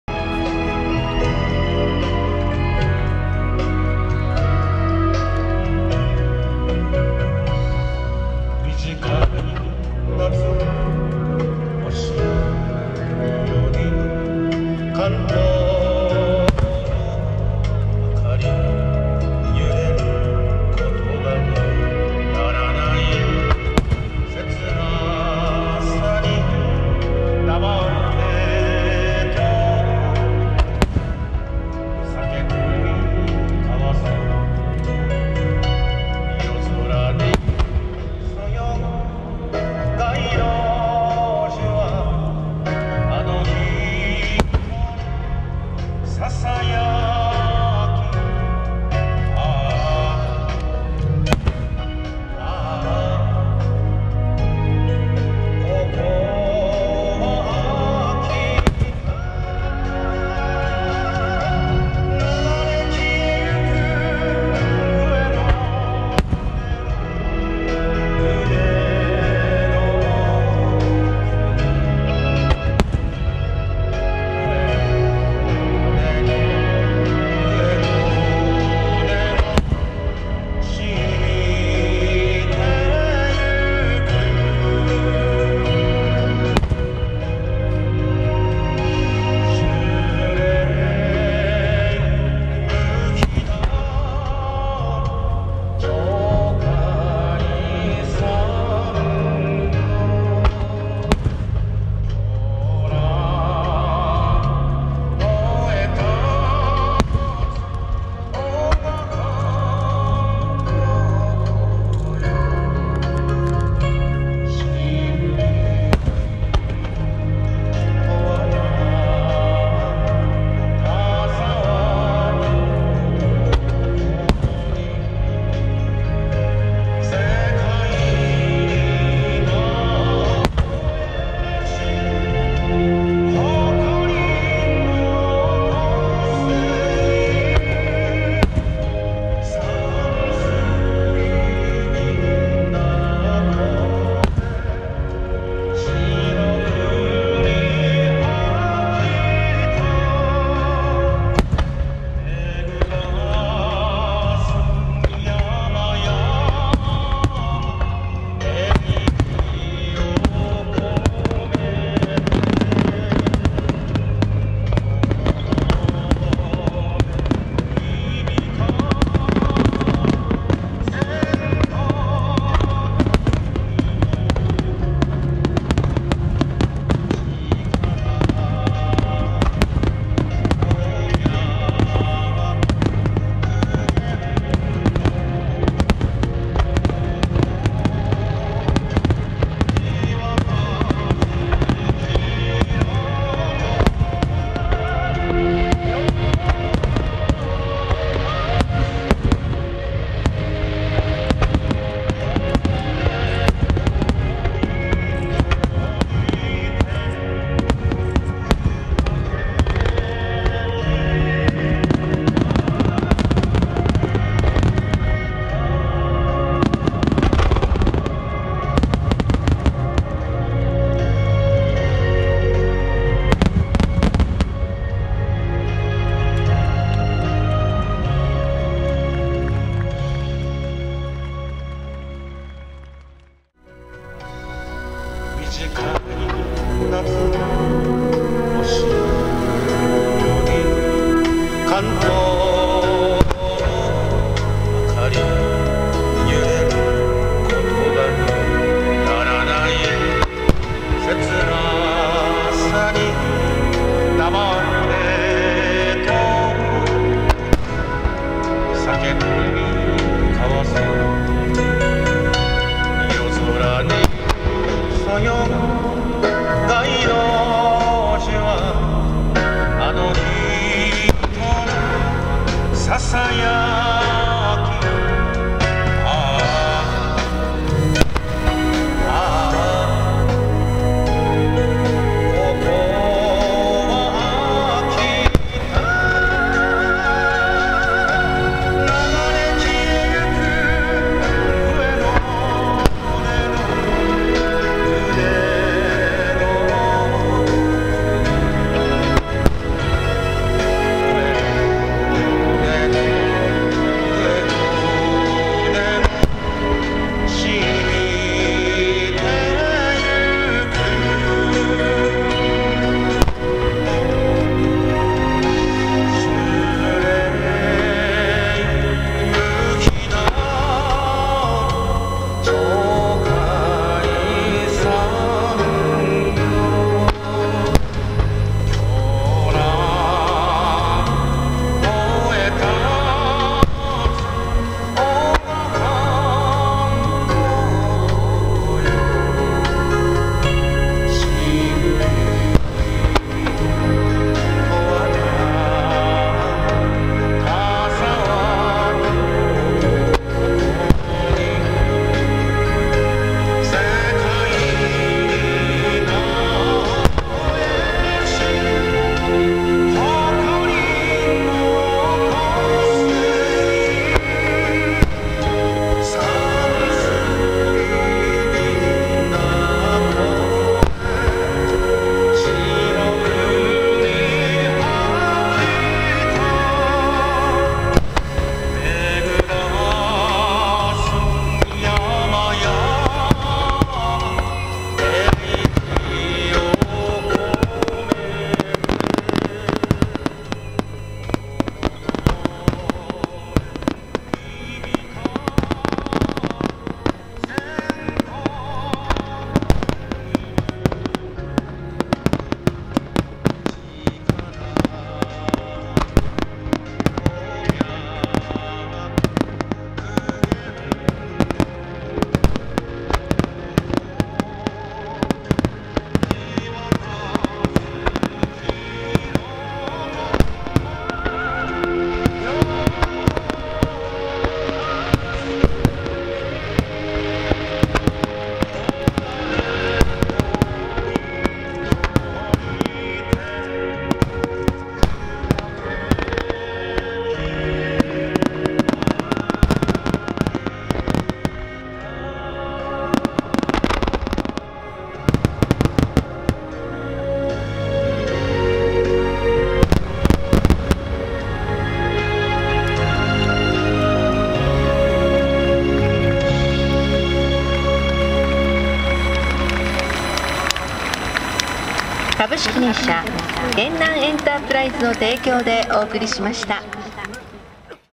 大曲の花火2019 全国花火競技大会 【フィナーレ・特大スターマイン】2019.8.31
前半は Goproで撮影。

後半 3:53 LUMIX で撮影しています。

[撮影機材]
Panasonic LUMIX G8
GoPro 6